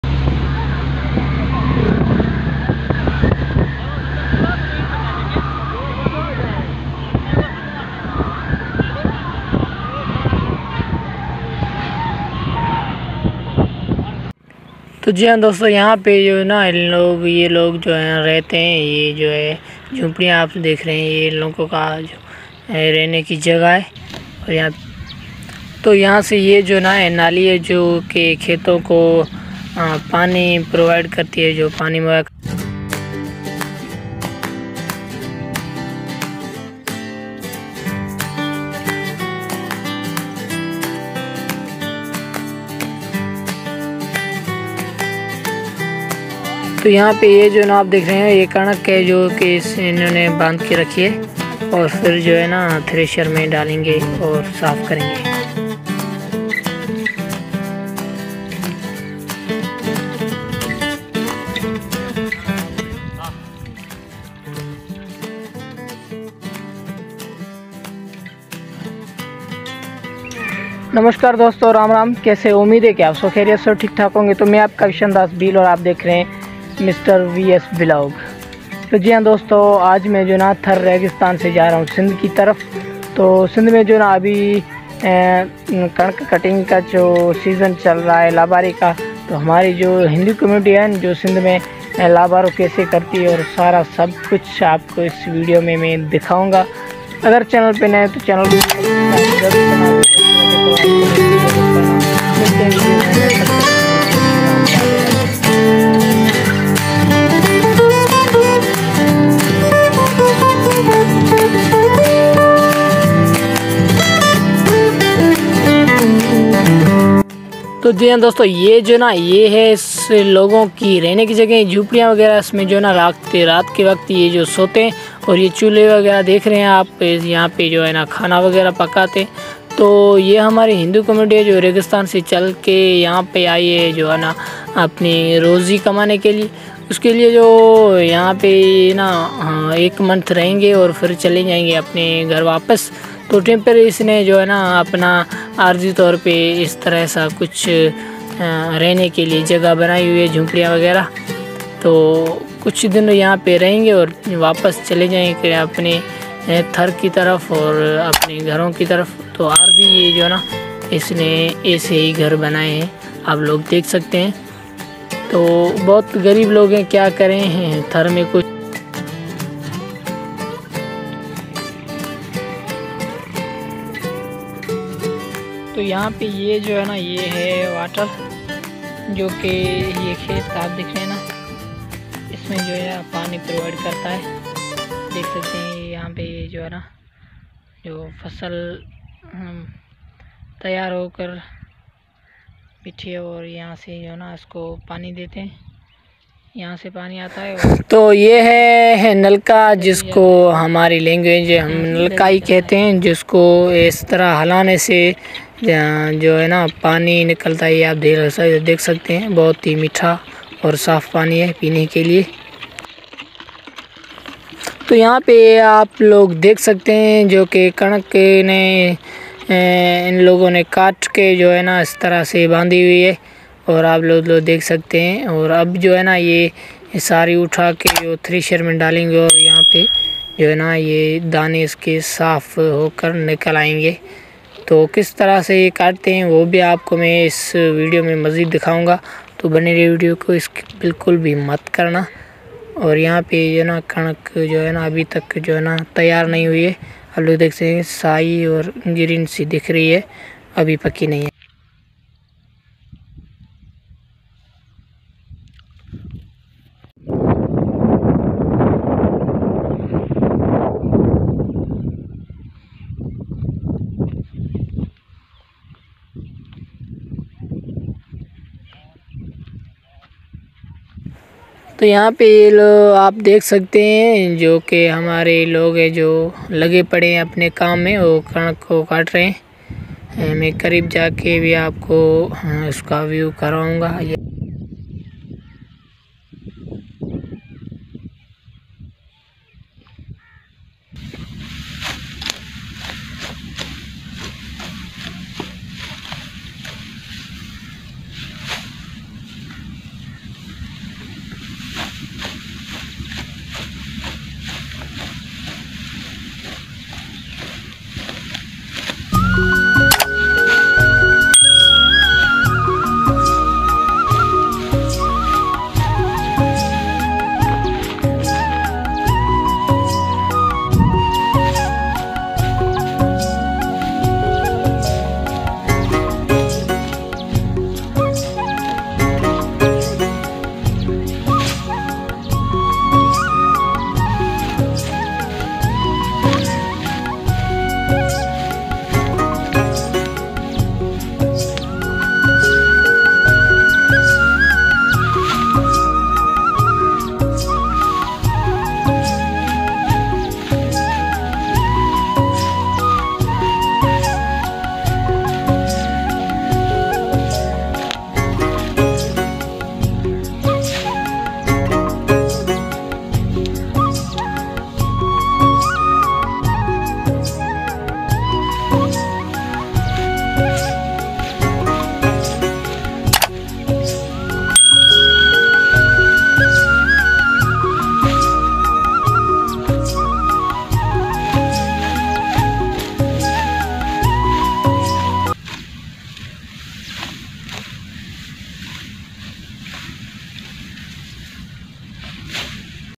पाकिस्तान सिंध में हिंदू मजदूरी करते हैं || Hindus Labours In Pakistan Sindh |Village Life in Sindh
Dhaniwad Dosto 🙏